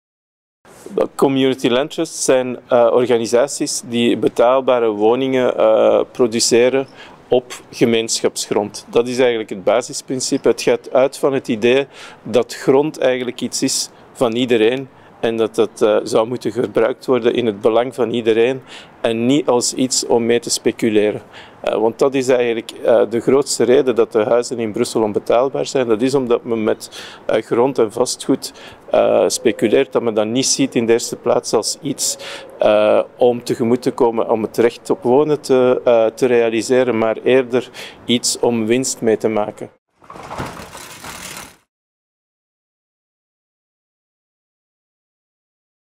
Geert - Campagne 2020 - Onbetaalbaar? Onaanvaardbaar!
We geloven in de mogelijkheid om erfpacht te gebruiken als manier om woningen betaalbaar te maken. Zoals Community Land Trust dat nu al doet in Brussel en Gent.

Met veel aandacht voor participatie van de bewoners, waar ook mensen met de laagste inkomens een kans krijgen.

Community Land Trust Brussel is daarom een campagneproject. Geert De Pauw, coördinator van Community Land Trust Brussel, getuigt: